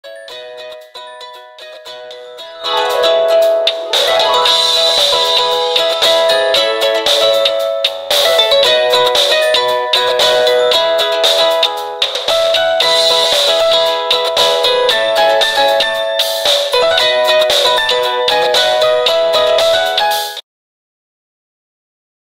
VinciBot Listening to Music LED Matrix can Vibrate According to the Volume of the Music
Code the LED matrix to vibrate according to the volume of the music.